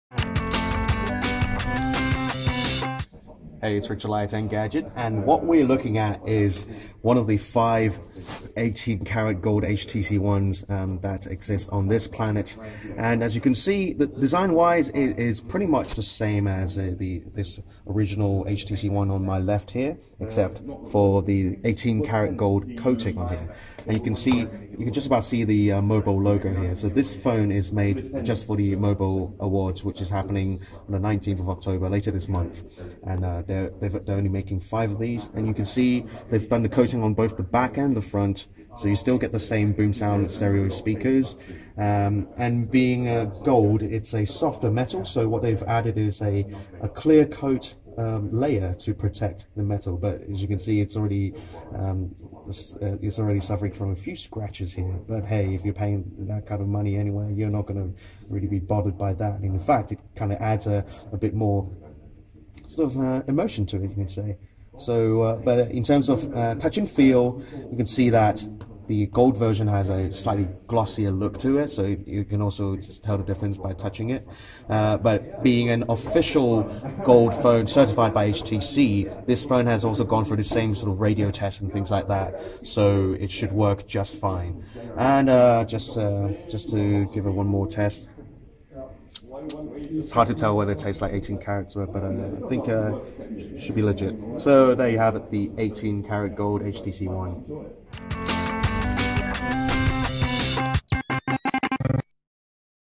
HTC One 18 Carat Gold Edition hands-on | Engadget
Engadget checks out (and licks) the 18 carat gold HTC One. It turns out that 18 carat gold is slightly too soft to be a sensible material for smartphones. We've been hands-on with one of just five MOBO-branded gold HTC Ones, which are evidently a lot less subtle than the gold iPhone 5s (shown top right). While the exclusive $4,500 handset definitely wasn't as heavy as we were expecting, its delicate surface had already suffered a multitude of tiny scratches by the time we picked it up -- and that's despite a special protective coating that doesn't seem to be helping much.